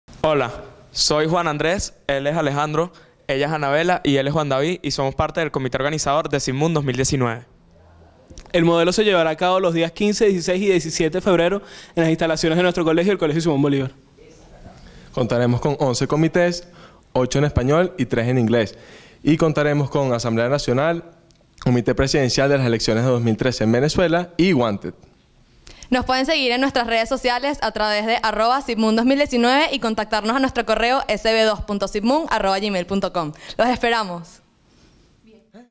SIBMUN 2019
Juan Andrés, Alejandro, Anabella y Juan David del Modelo de Naciones Unidas del Colegio Simón Bolívar, los invitan a participar en SIBMUN. El Modelo se realizará el 15, 16 y 17 de febrero.